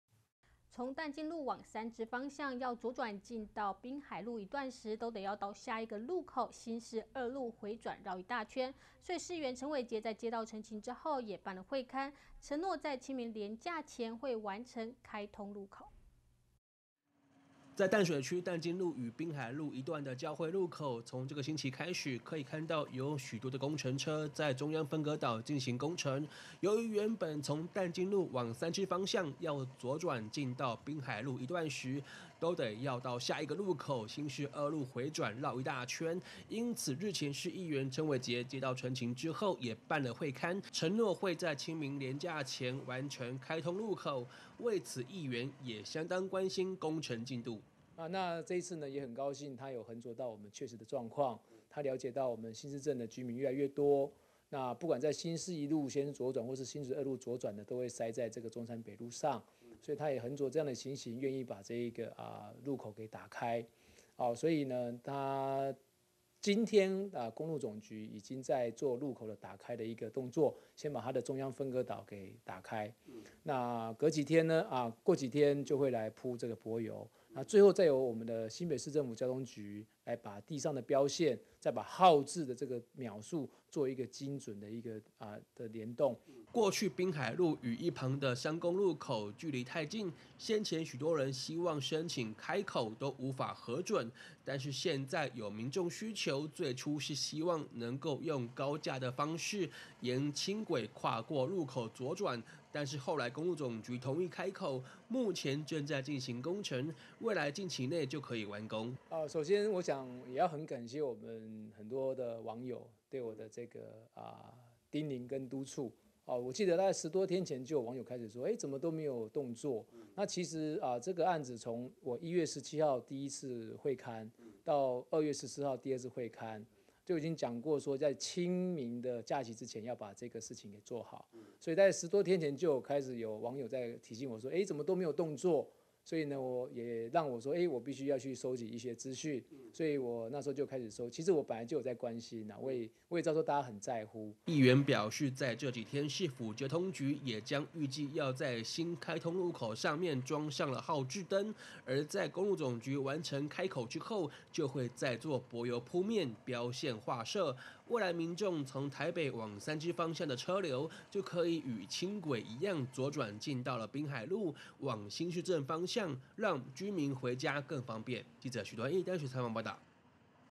左轉進濱海路開口 議員工程將完工啟用
在淡水區淡金路與濱海路一段的交會路口，從這個星期開始，可以看到有許多工程車在中央分隔島進行工程，由於原本從淡金路往三芝方向要左轉進到濱海路一段時，都得要到下一個路口新市二路回轉繞一大圈，因此日前市議員陳偉杰接到陳情之後，也辦了會勘承諾在清明連假前會完成開通路口，為此議員也相當關心工程進度。

市議員 陳偉杰

議員表示，過去濱海路與一旁的商工路口距離太近，先前許多人希望申請開口，都無法核準，但是現在有民眾需求，最初是希望能夠用高架的方式，沿輕軌跨過路口左轉，但後來公路總局同意開口，目前正在進行工程，未來近期內，就可完工。

市議員 陳偉杰

議員表示，在這幾天市府交通局也將，預計要新開通路口裝上號誌燈，而在公路總局完成開口後，就會再做柏油鋪面，標線劃設，未來民眾從台北往三芝方向的車流，就可以與輕軌一樣左轉進到濱海路，往新市鎮方向進入，讓居民回家更方便。